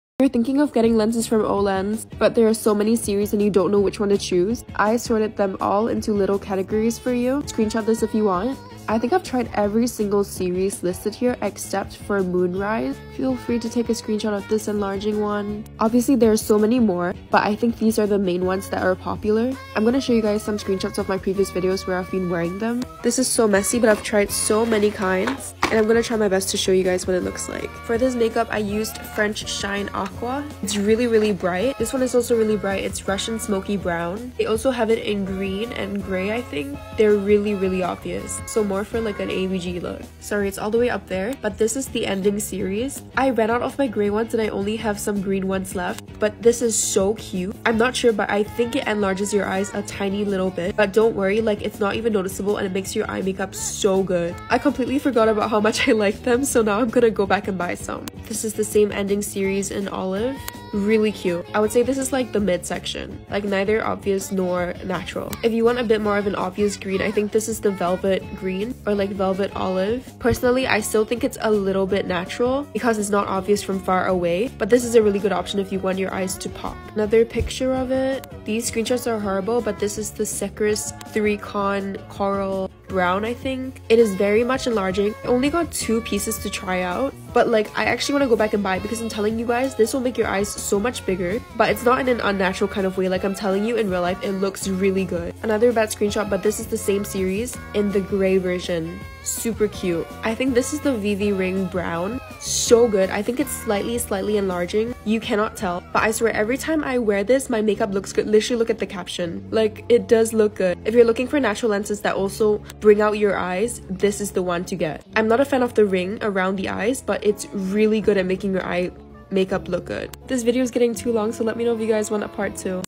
[ 𝗢𝗟𝗘𝗡𝗦 𝗖𝗢𝗟𝗢𝗨𝗥𝗘𝗗 𝗟𝗘𝗡𝗦𝗘𝗦 𝗚𝗨𝗜𝗗𝗘 _𝗳𝘁@cwassaunt❤️]
"So many series and you don't know what to choose,
pls screenshot my OLENS categories"
Struggling to find the ultimate style of contacts for your eyes??
Hope this helped you guys know what it looks like and what vibe u wanna go for!